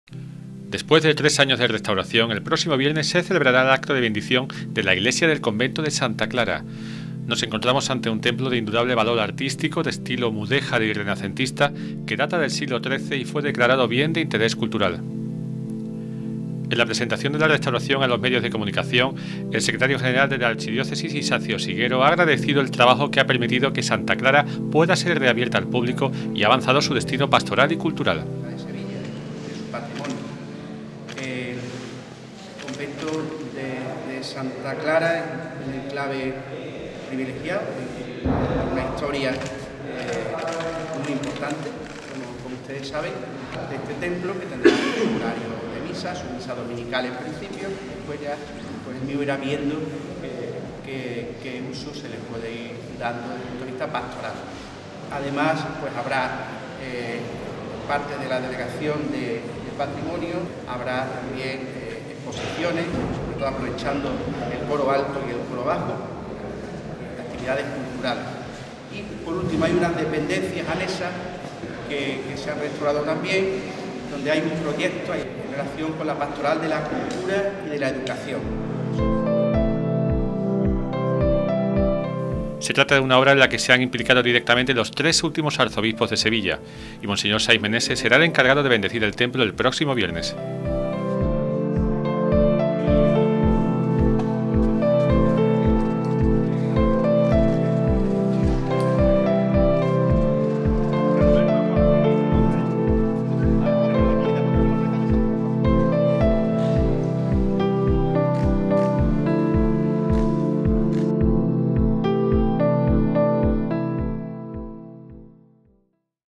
Presentación de la restauración de Santa Clara
La Archidiócesis de Sevilla ha presentado a los medios de comunicación el resultado de las obras de restauración de la iglesia del convento de Santa Clara, en Sevilla. Su bendición, a cargo de monseñor Saiz Meneses, se llevará a cabo la mañana del 10 de febrero de 2023.